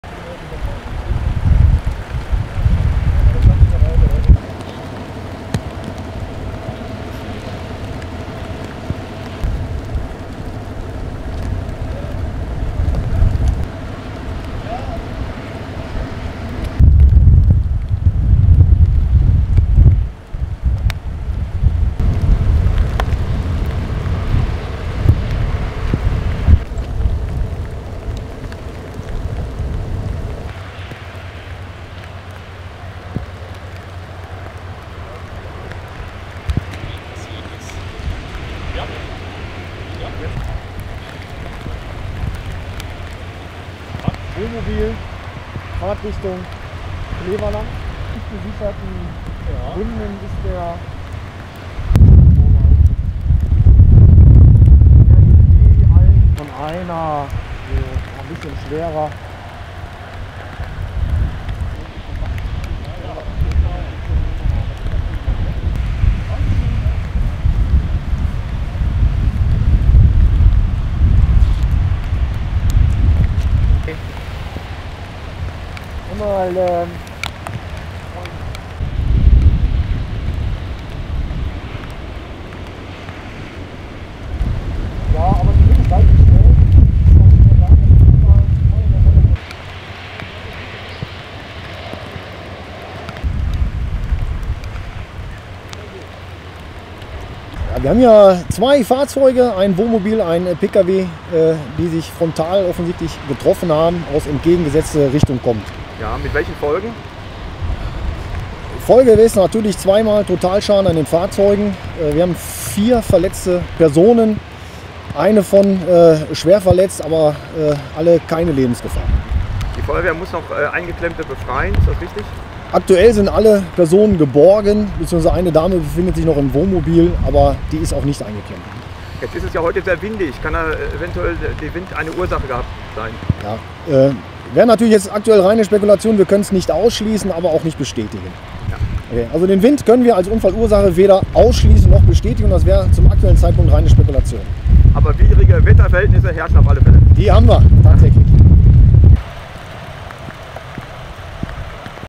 BMW kracht in Wohnmobil - Einsatz für die Feuerwehr Sonsbeck
Am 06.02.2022, gegen 12:00 Uhr, ereignete sich auf der Achterhoeker Straße, in Höhe der Autobahnüberführung der Bundesautobahn 57, ein schwerer Verkehrsunfall. Ein 24-jähriger Mann aus Sonsbeck befuhr mit seinem Pkw, BMW 3er, den Achterhoecker Schulweg in Fahrtrichtung Sonsbeck. Nach Durchfahren der Autobahnüberführung der Bundesautobahn 57 verlor der 24-jährige BMW-Fahrer auf der Achterhoecker Straße, aus bisher ungeklärter Ursache, die Kontrolle über seinen Pkw und geriet auf die Gegenfahrbahn. Hier kam es zu einem Frontalzusammenstoß mit dem entgegenkommenden Fiat Wohnmobil eines 58-jährigen Mannes aus Brüggen. Die beiden beteiligten Fahrzeugführer sowie eine 56-jährige Frau aus Brüggen, welche sich als Beifahrerin in dem Wohnmobil befunden hatte, wurden durch den Zusammenstoß schwer, jedoch nicht lebensgefährlich, verletzt und mussten mit angeforderten Rettungswagen örtlichen Krankenhäusern zugeführt werden. An den beteiligten Fahrzeugen entstand ein geschätzter Gesamtsachschaden in Höhe von etwa 20.000,-EUR. Beide Fahrzeuge wurden sichergestellt und mussten abgeschleppt werden. Für den Zeitraum der Verkehrsunfallaufnahme musste der Achterhoeker Schulweg, sowie die Achterhoecker Straße im Bereich der Unfallstelle bis um 14:52 Uhr komplett für den Fahrzeugverkehr gesperrt werden. Die Ermittlungen zur Unfallursache dauern noch an.